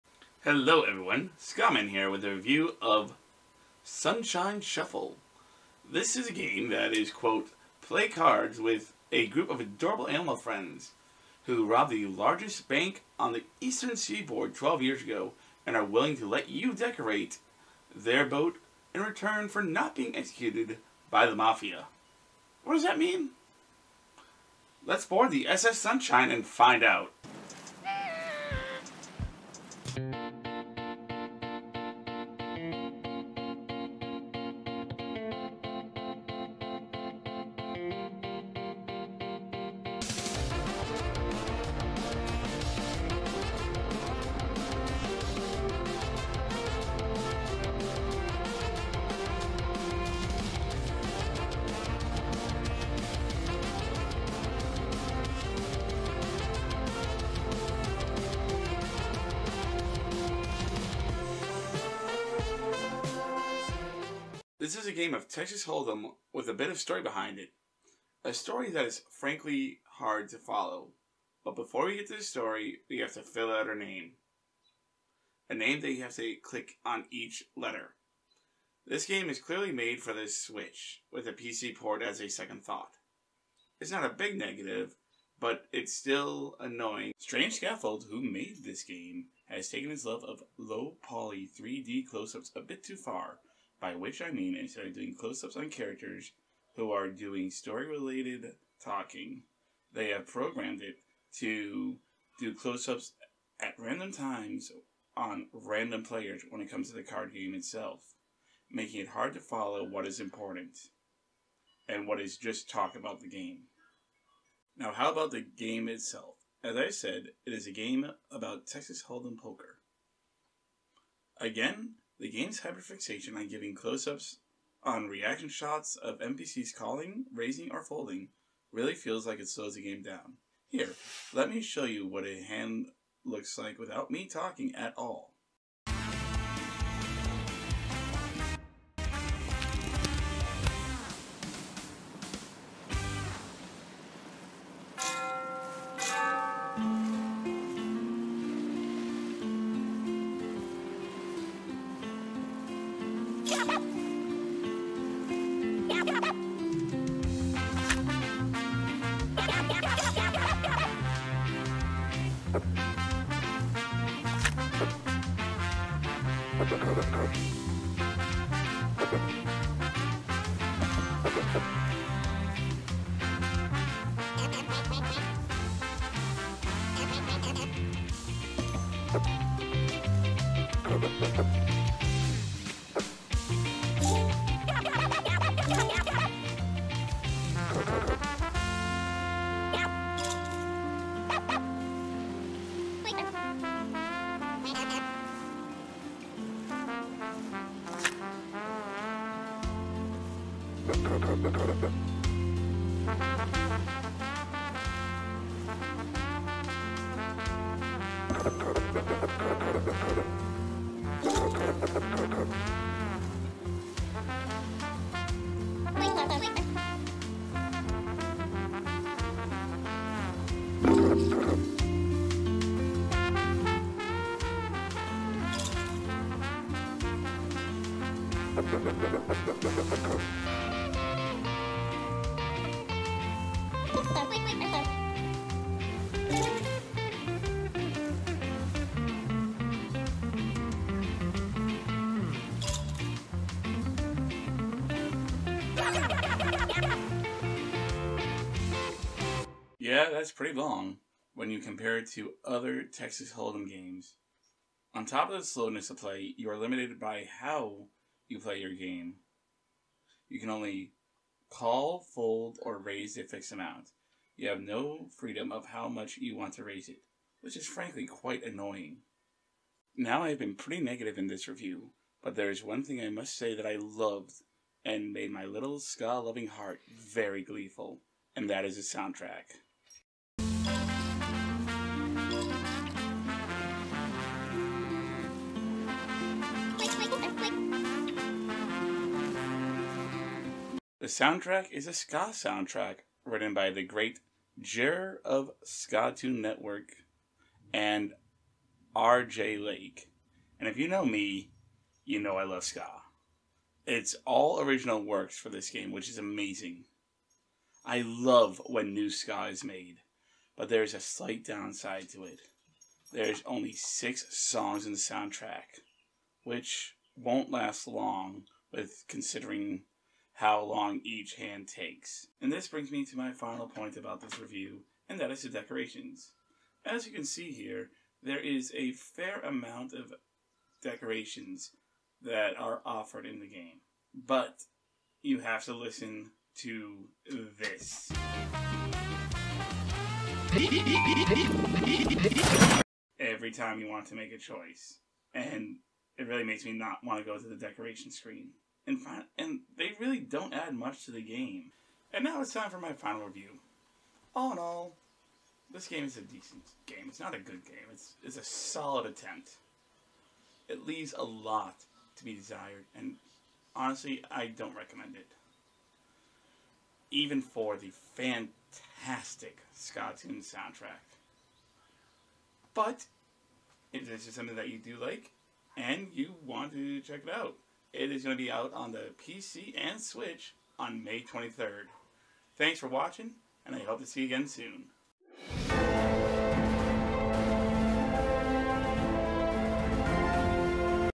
A review of Sunshine Shuffle
This is my review of the new game Sunshine Shuffle coming out on May 23rd for PC and Switch.